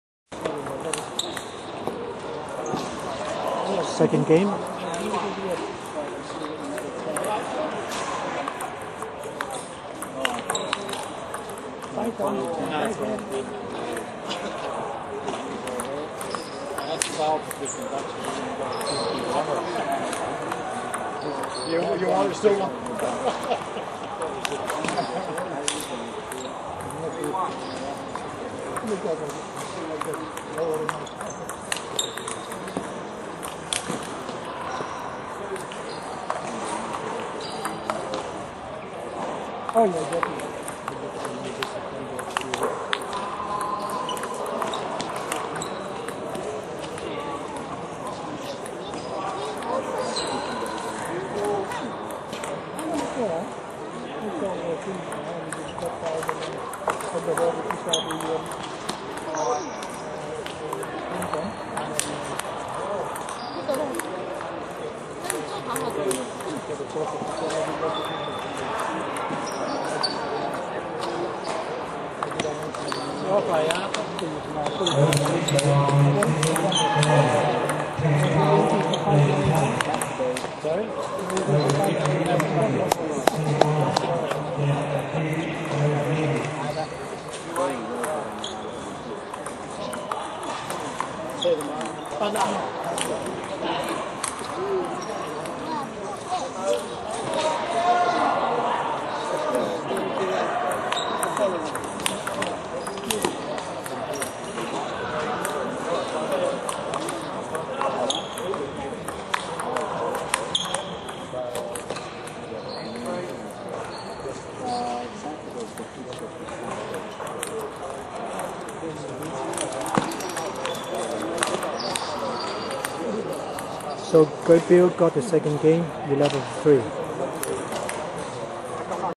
2010 NSW Vet Table Tennis Open Champ, O/50 Men's SF, Bill vs Truong, 2nd set!
2010 NSW Vet Table Tennis Open Champ, O/50 Men's SF, Bill Robson vs Truong, 2nd set